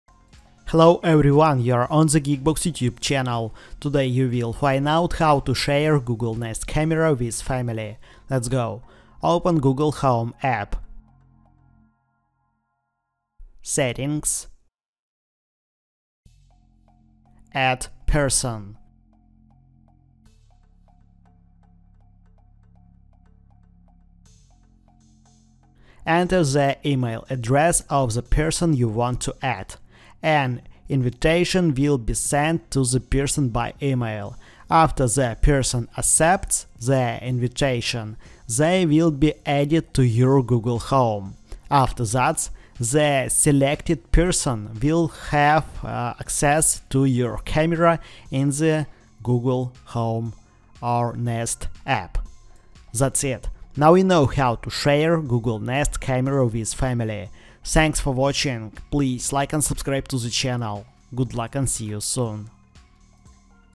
How To Share Google Nest Camera With Family
How To Share Google Nest Camera With Family 2024
Hello everyone, you are on the geekbox YouTube channel
Today you will find out How To Share Google Nest Camera With Family
Let's go
open google home app
settings
add person
An invitation will be sent to the person by Email
After the person accepts the invitation, they will be added to your google home
After that, the selected person will have access to your camera in the Google Home or Nest app
That's it, now you know How To Share Google Nest Camera With Family